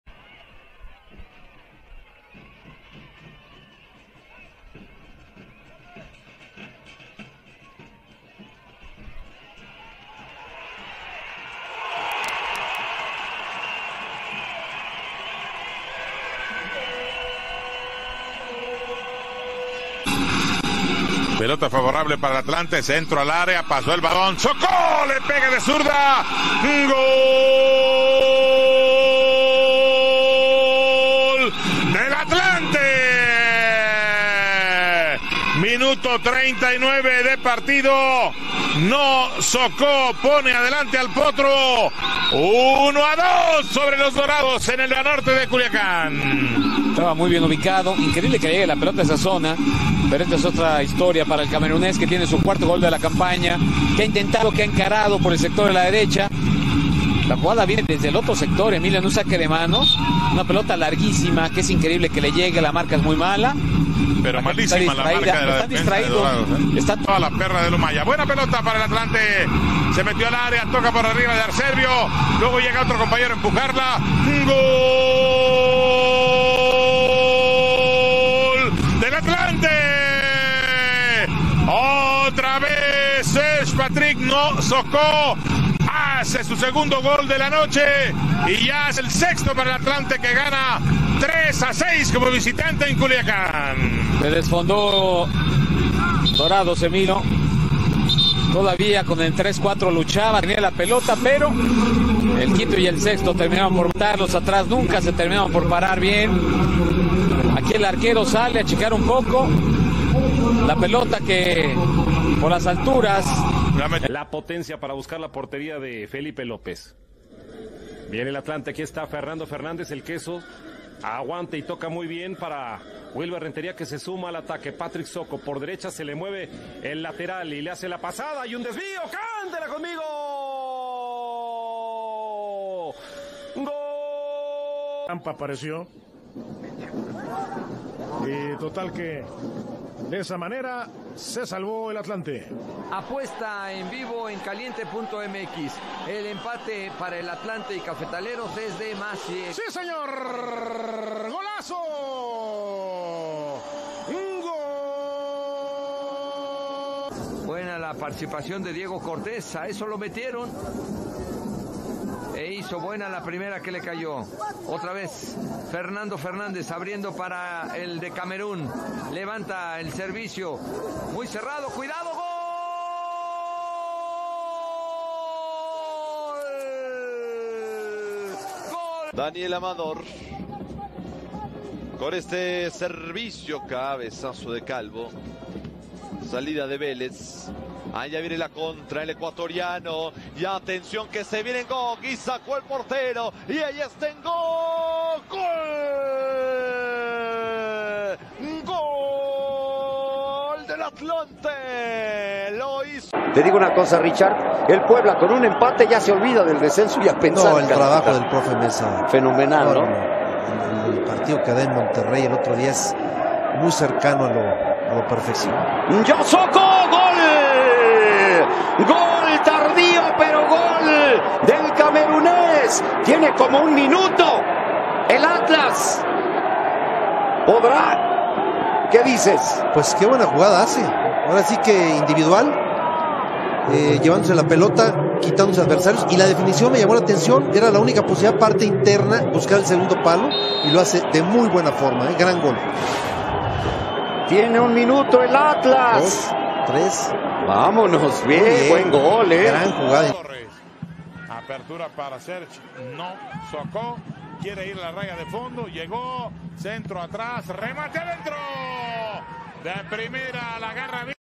Patrick Soko🇨🇲 Goles y Asistencias (2017-2020)
Vídeo exclusivo de Patrick Soko, nuevo futbolista del Racing. ¡Quédate hasta el final!